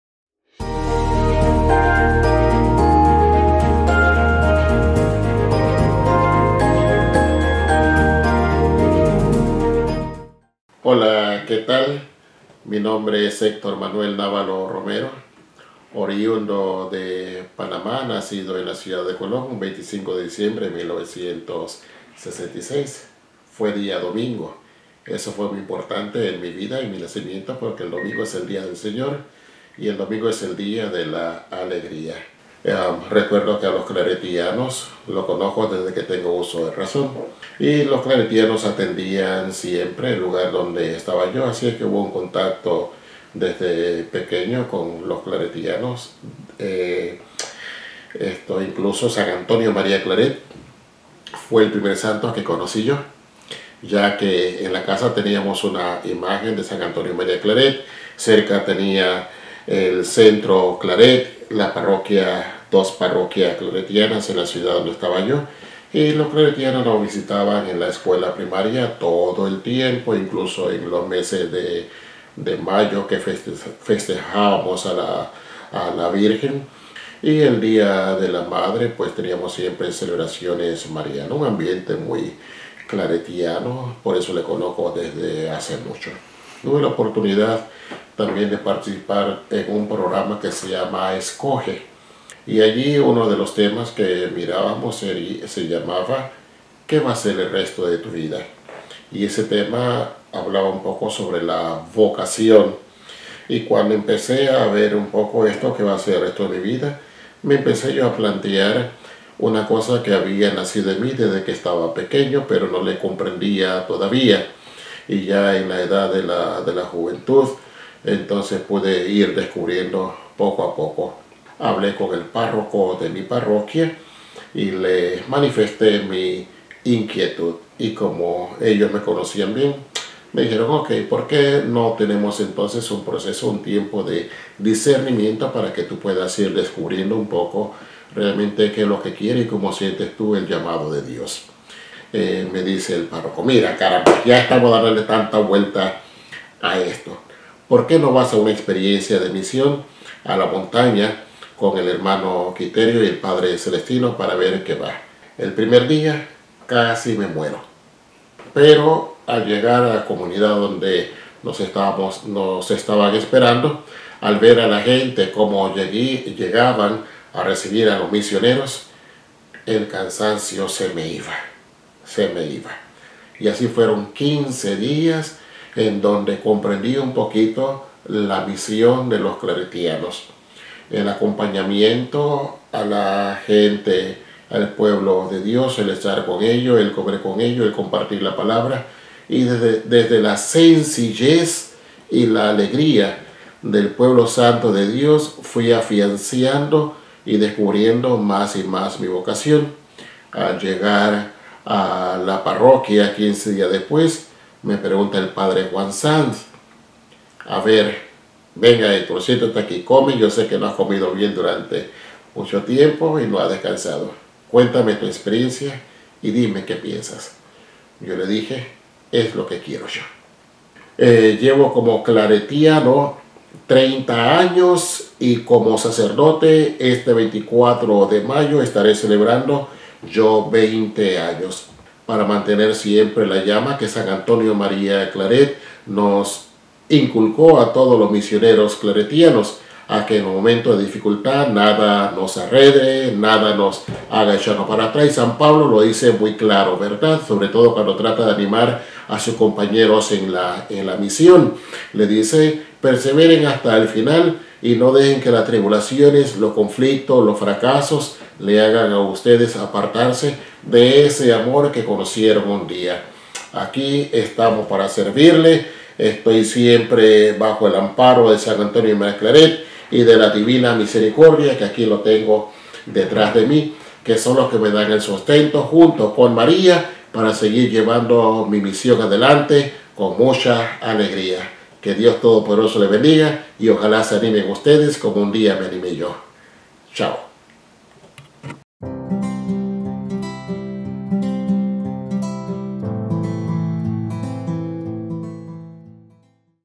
When I was a child my dream was to become a missionary
Fr. Hector grew up surrounded by Claretians in his native Panama.  They were involved in every ministry in his community and had great admiration for their missionary spirit.   He recalls that as I child he said, "This is what I want for me, I want to be a missionary."  Later, as a young adult and after a couple years of discernment, his pastor challenged him to really think about his vocation.  He did and said "yes" to God!